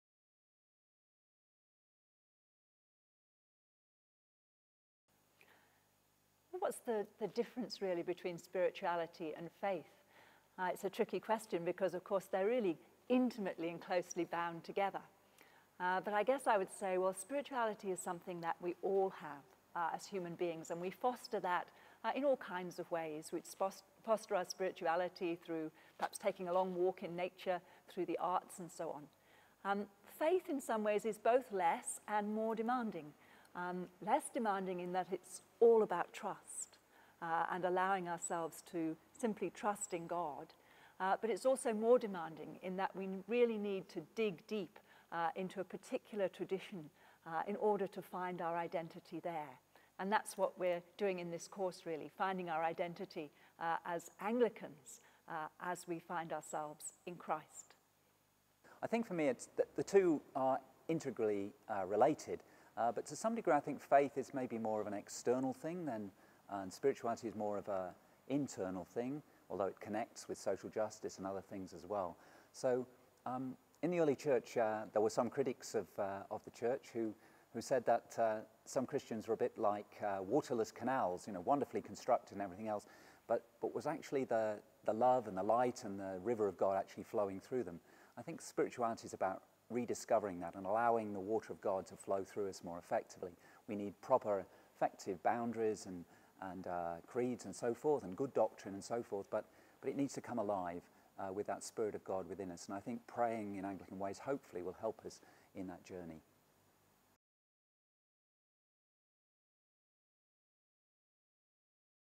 FAITH360: Praying in Anglican Ways FAQ 4. What’s the difference between spirituality and faith?
FAITH360: Praying in Anglican Ways is a workshop that opens up a number of interesting questions about prayer.  In this series of Frequently Asked Questions, co-authors Jonathan Inkpin and Penny Jones tackle some in a thoughtful and orderly way.

FAITH360: Praying in Anglican Ways is a module from The 360 Project, an educational program designed by St Francis Theological College to enable faithful people to engage with issues of faith, theology and everyday life.